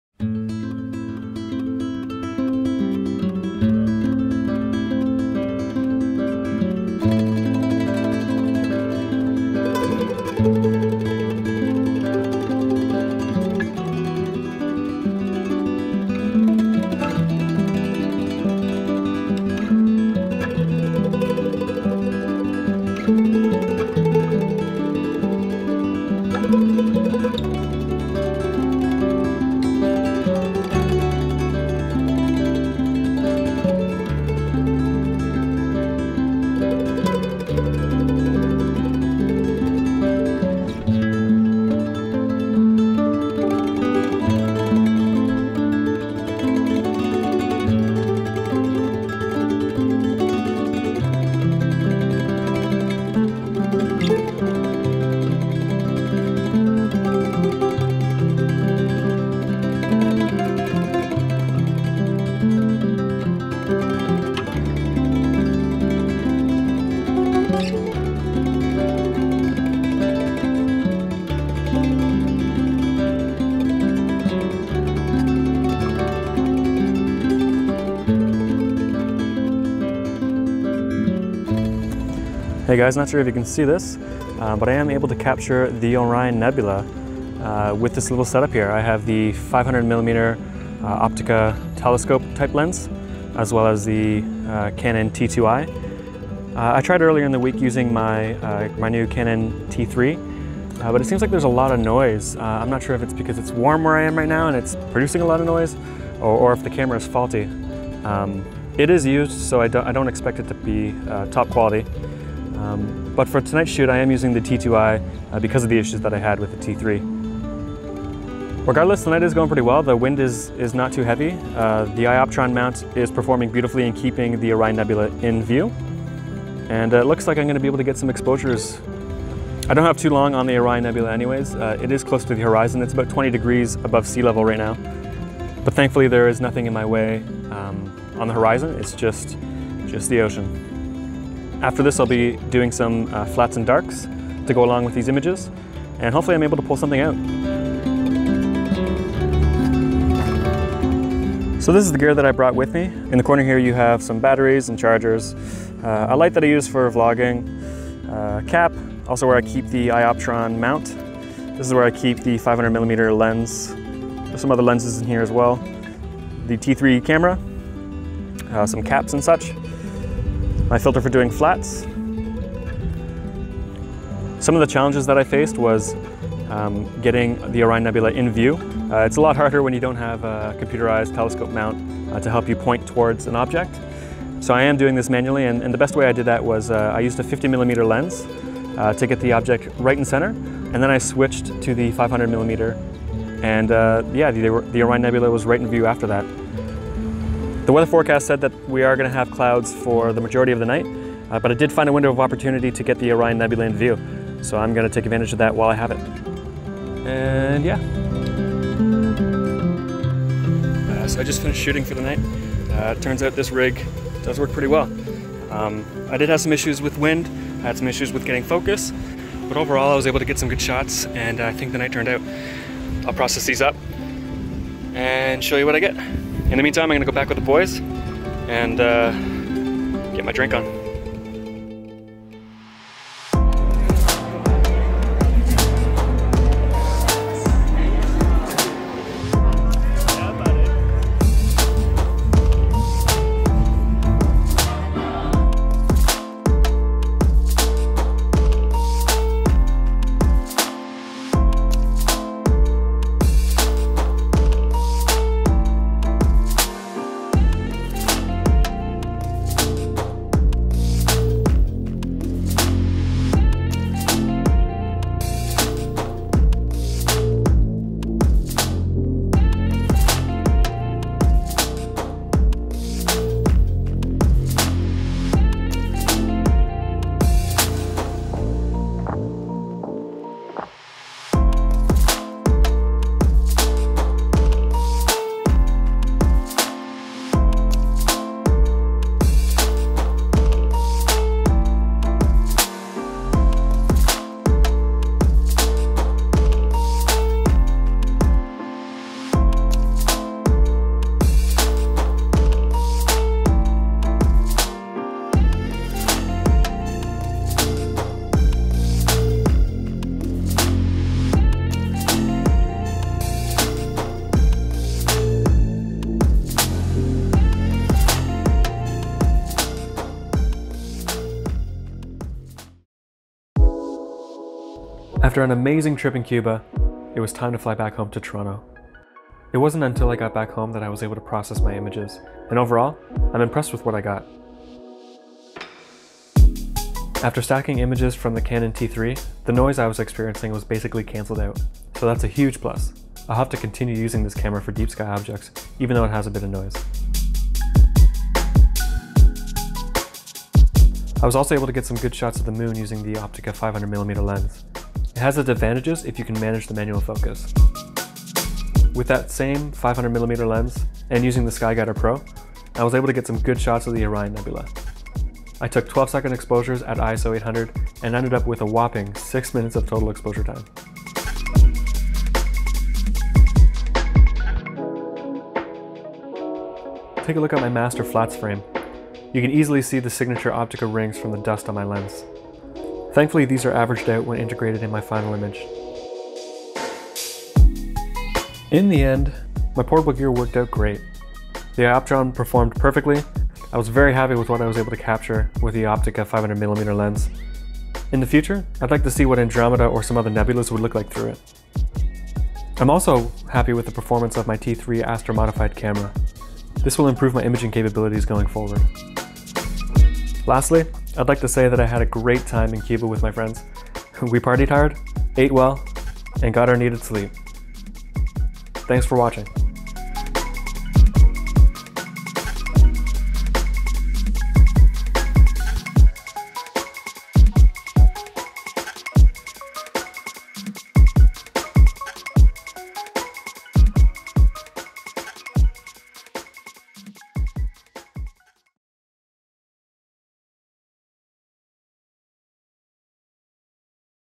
Astrophotography On The Go (In Cuba) - Celestial Photography Vlog #8
In this vlog, I flew down to Cuba to celebrate my friends bachelor party. I was able to sneak away from the drinking to get some great astrophotography shots using the skyTracker Pro, Opteka 500mm lens and my new astro-modified canon T3 camera. It was a great trip overall, and im happy with what I was able to capture with my portable setup.